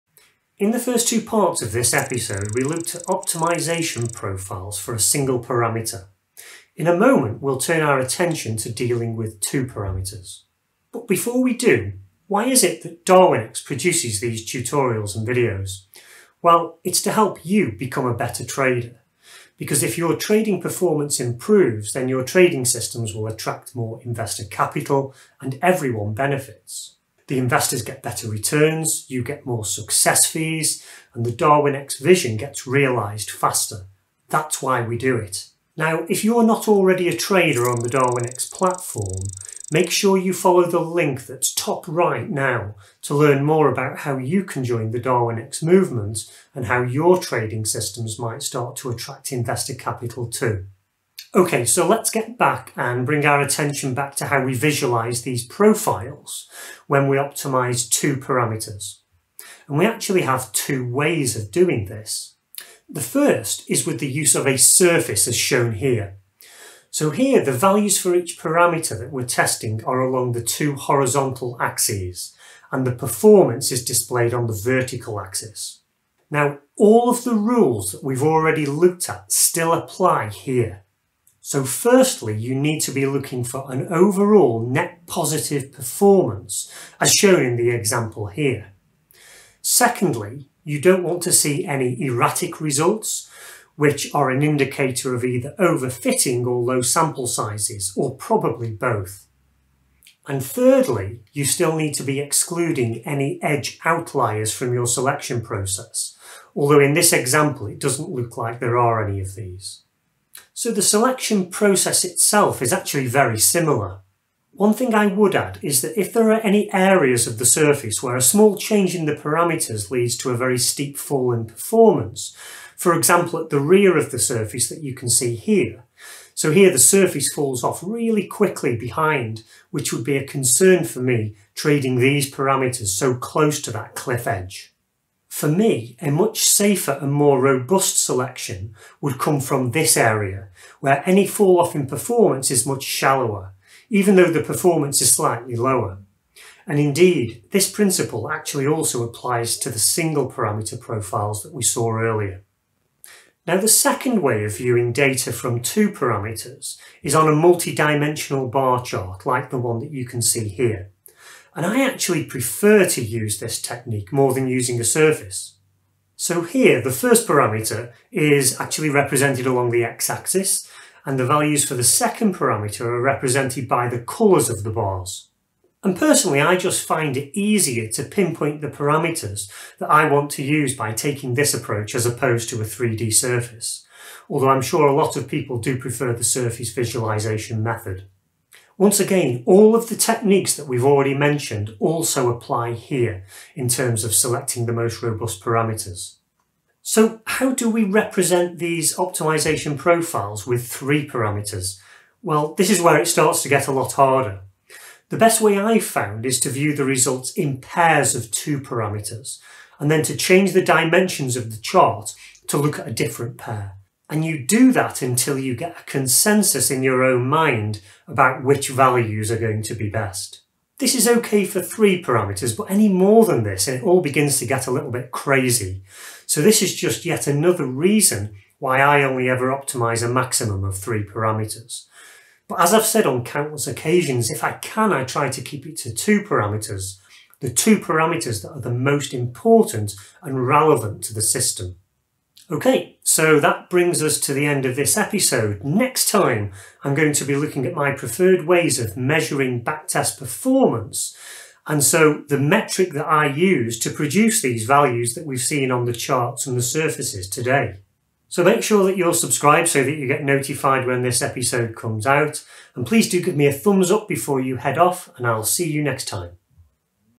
8.3) Using 3D Optimization Surfaces to Ensure Robust Parameter Selection | Algorithmic Backtesting
Are you using optimization profiles to effectively select the most robust parameters from your algorithmic trading optimizations? There are several major pitfalls that you need to be aware of and avoid to ensure you don't select ineffective parameters.

YouTube’s algorithms measure the quality of Darwinex content on the basis of:
- Reach
- Engagement

With seemingly small actions such as:
- etc